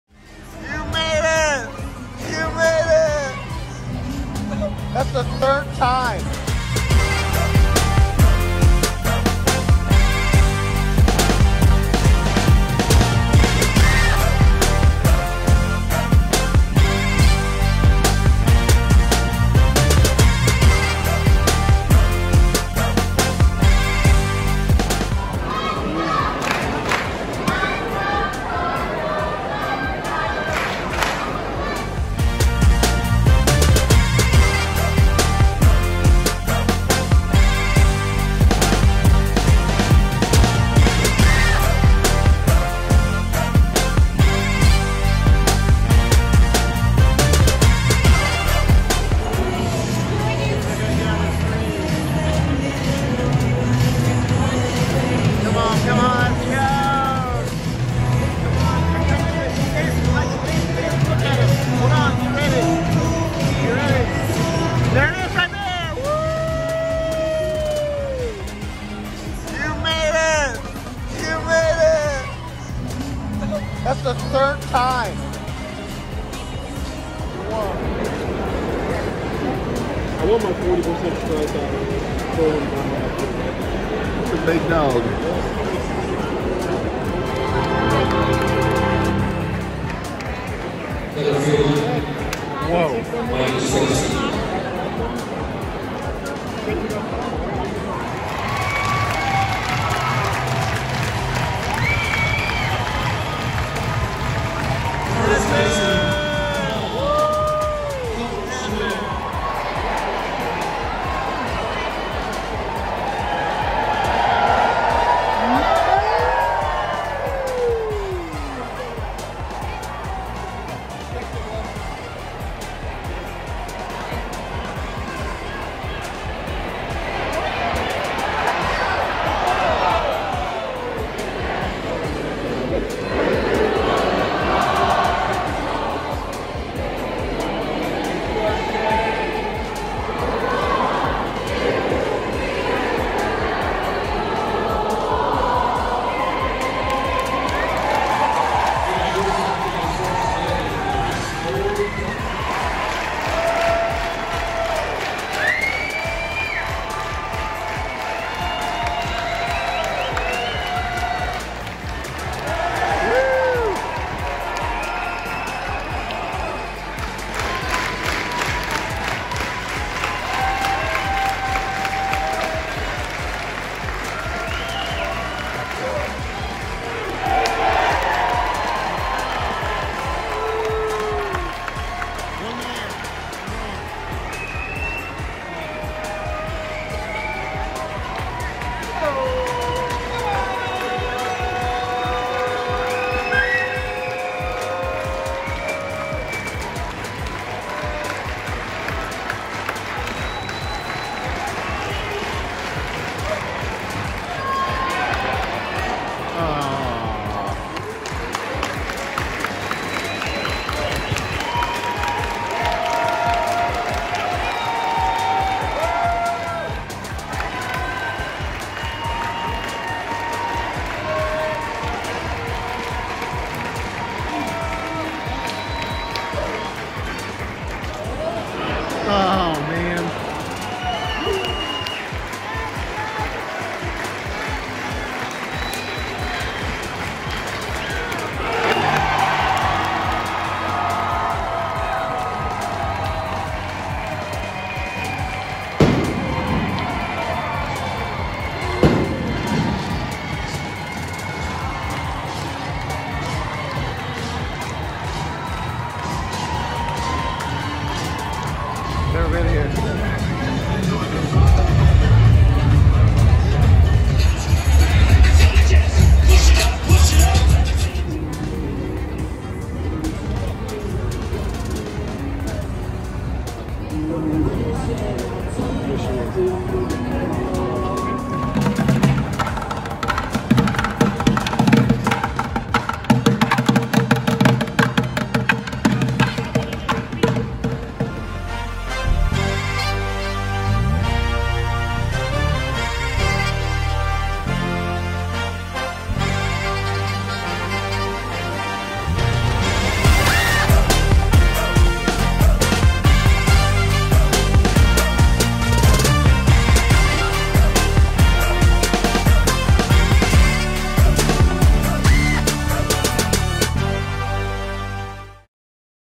CARDINALS WIN!!!
The whole family went to a cardinals game!